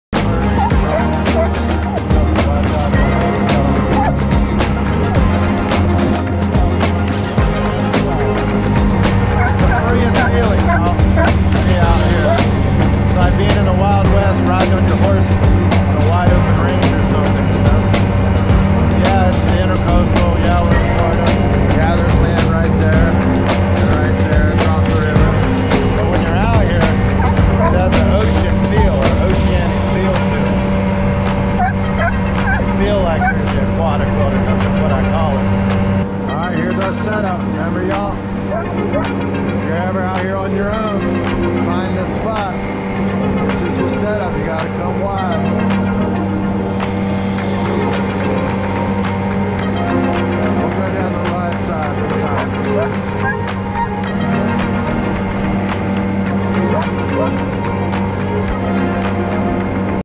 Belgian Malinois Tiny Boat¹⁰😄
Out on the speeder my 8ft Jonboat riding the river with Gypsea my Belgian Malinois dog. We are chasing the sun. lovin the life here in Sebastian, Florida. belgianmalinois malinois doglover boating saltlife jonboat awesome water maligator cool floridalife @CaptainNoBeard777